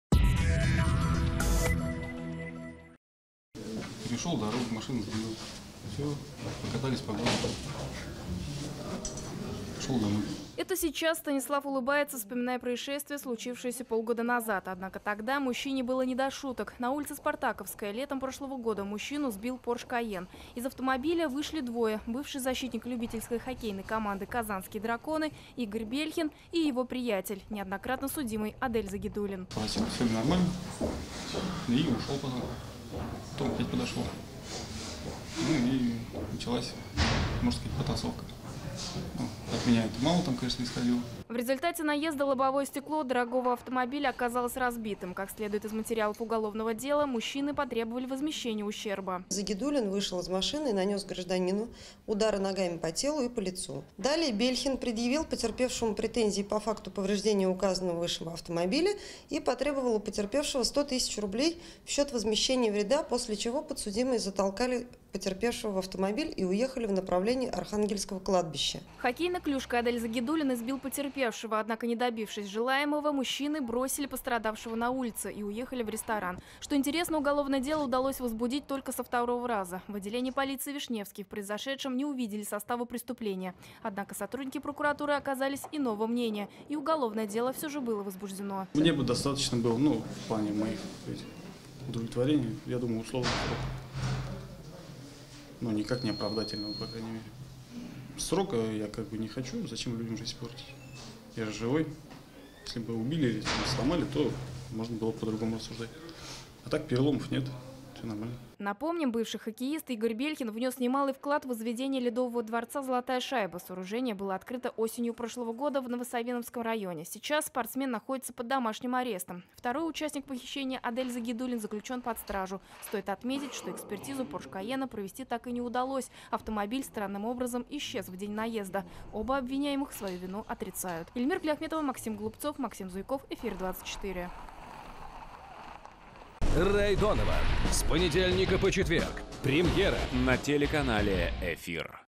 Мужчина и его приятель, наехав на пешехода, увезли его на кладбище и избили
В Вахитовском районном суде Казани состоялось первое заседание по делу о похищении и вымогательстве.